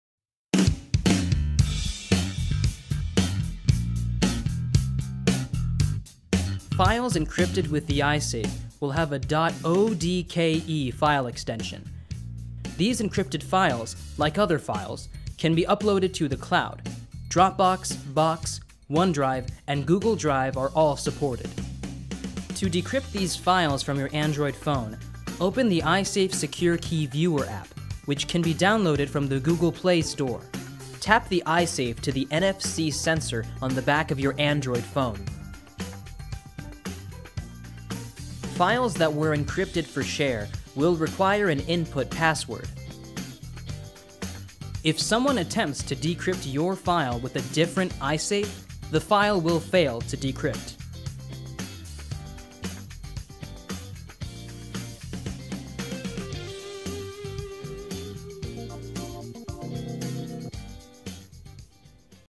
SUNIX iSafe - 如何使用iSafe解密與雲端分享
如果有人試圖要用其他iSafe來解密您的檔案時，是無法成功被解密的